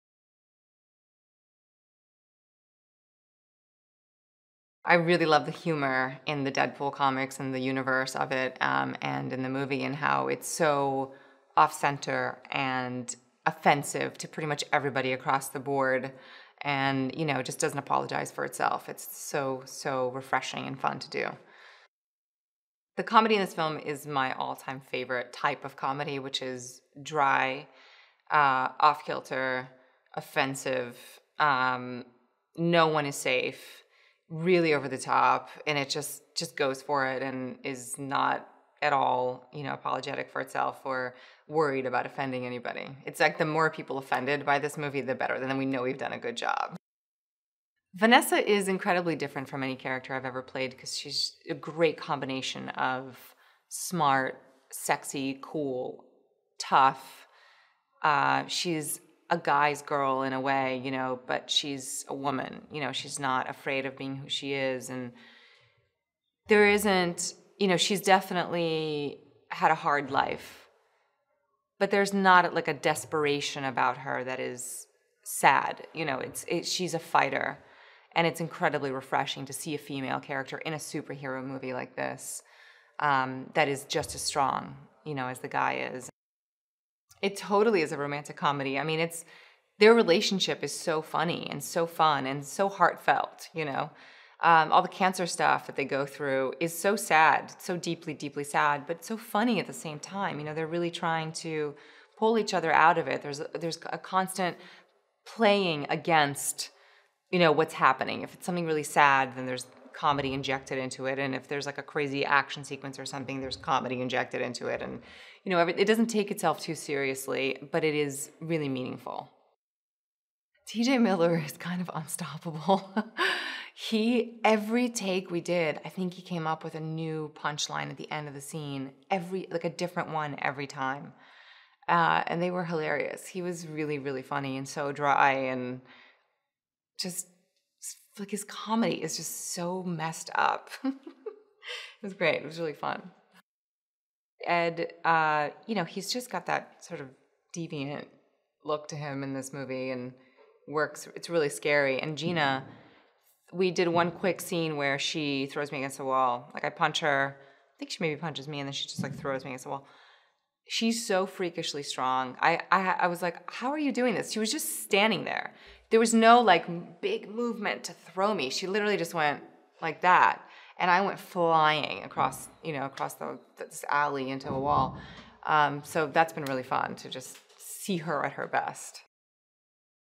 Deadpool Interview - Morena Baccarin (2016) - Action Movie HD
A former Special Forces operative turned mercenary is subjected to a rogue experiment that leaves him with accelerated healing powers, adopting the alter ego Deadpool.